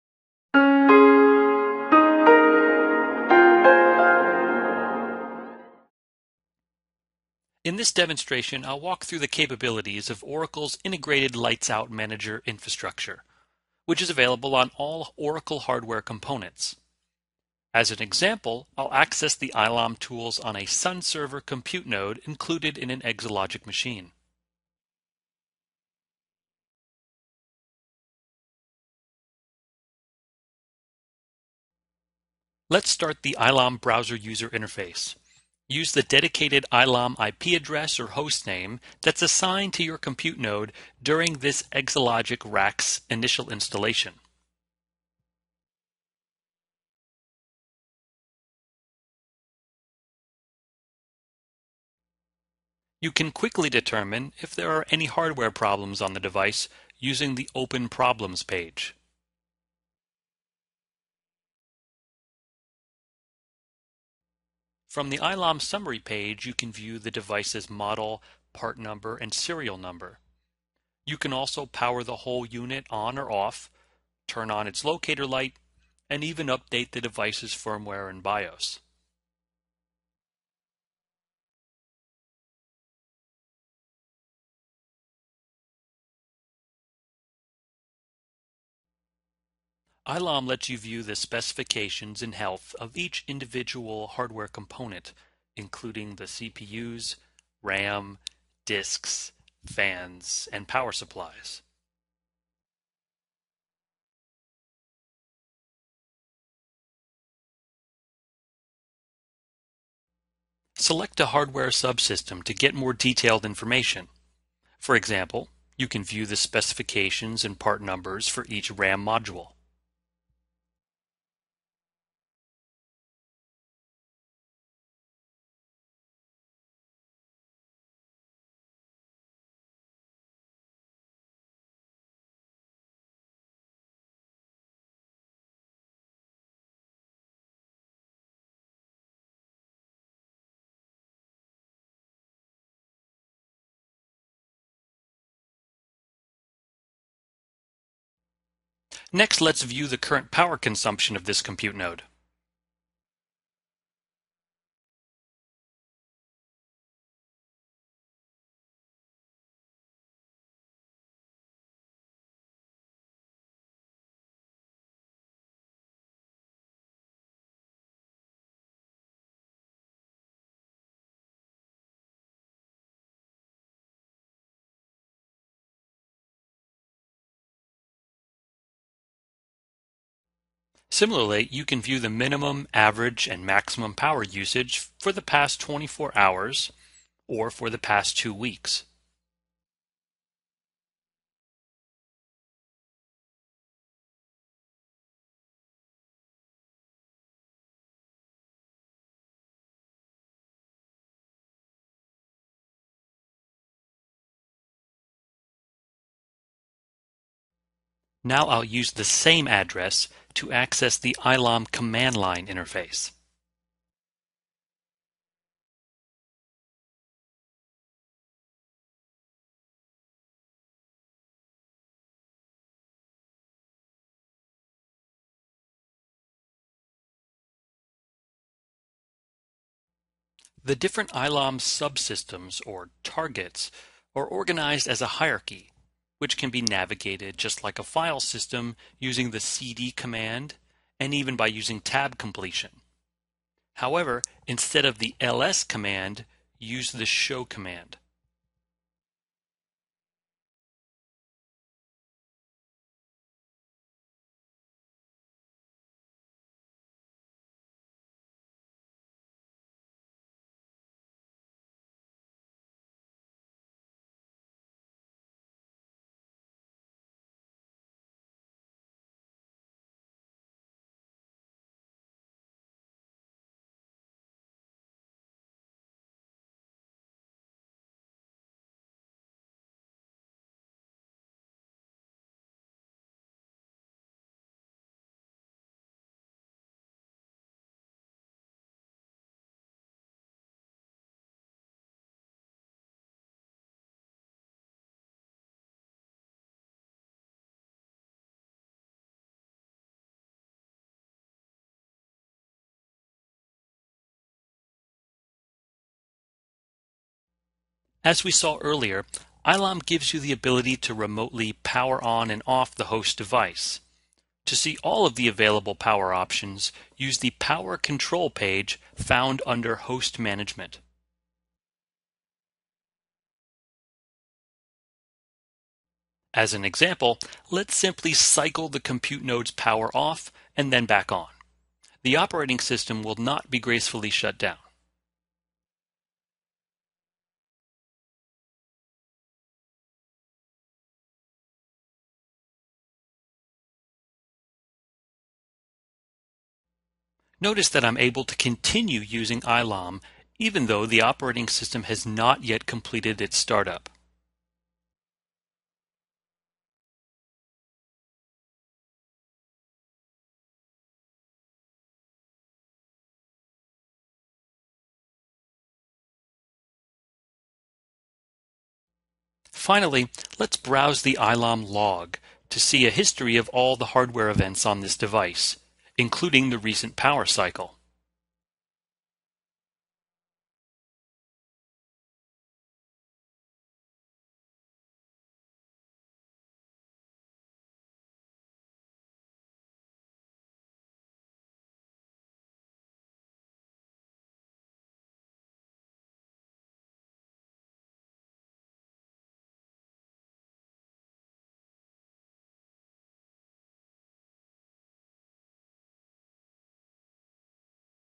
Exalogic Integrated Lights Out Manager (ILOM)
This recorded demonstration walks through the capabilities of Oracle's Integrated Lights Out Manager infrastructure, which is available on all Oracle hardware components. As an example this demo accesses the ILOM tools on a Sun Server compute node included in an Exalogic machine.